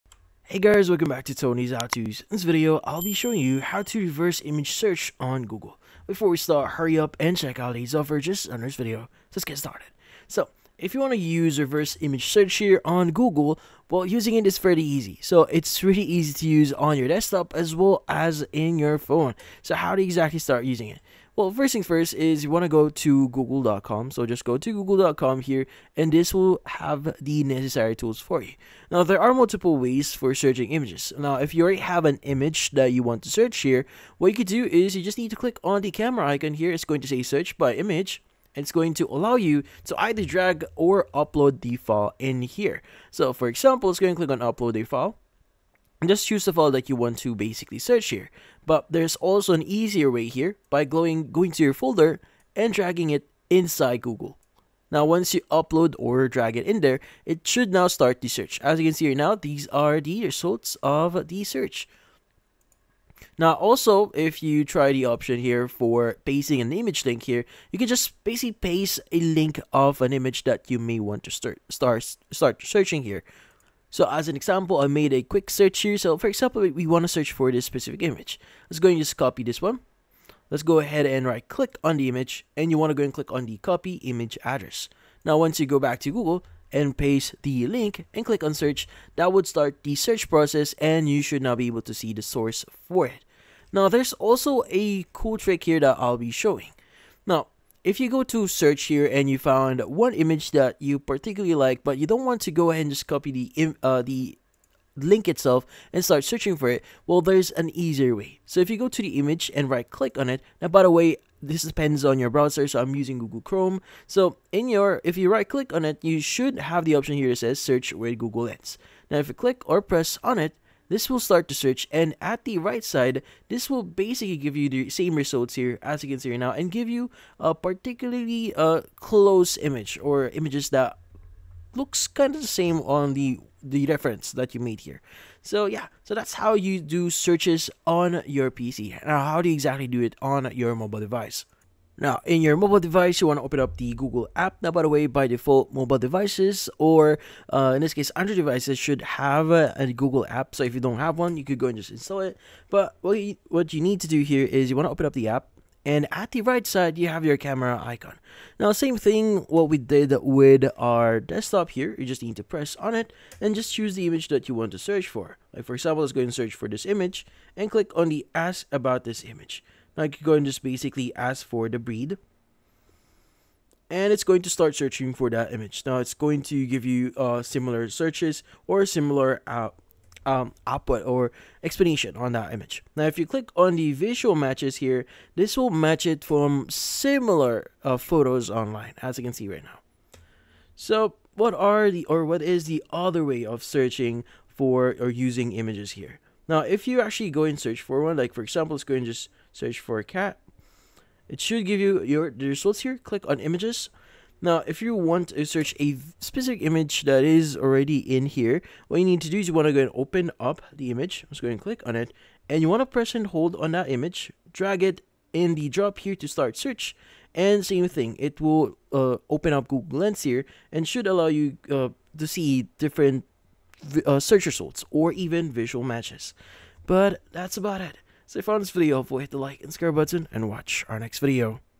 How to Reverse Image Search on Google (2026)
In this video, I show you How to Reverse Image Search on Google in 2026. The quick and easy tutorial best reverse image search engines free. Do you want to know step by step how to find people by photo on google in 2026? Yes? Great!